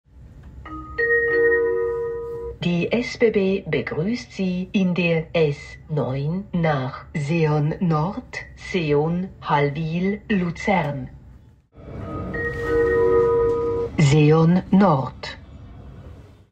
SBB Fragmente • S9 • Begrüssung Lenzburg – Luzern (Seon Nord)
Seit Fahrplanwechsel für 2023 hält die S9 Lenzburg – Luzern an der neuen Haltestelle Seon Nord in Seon. Wir gingen eigentlich davon aus, dass die TTS Stimme eingeführt wird, doch dem war nicht so. Also konnten wir noch "Seon Nord" ergattern.